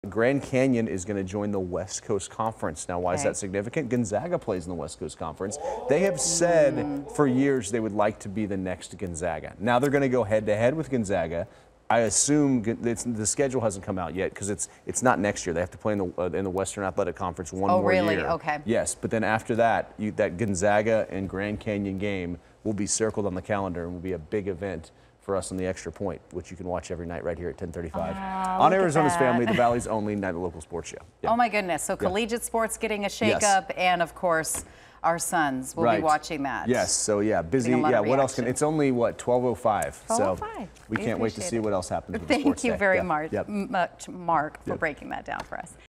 GCU to join West Coast Conference next year
Grand Canyon University has accepted an invitation to join the West Coast Conference starting in the 2025-26 academic year.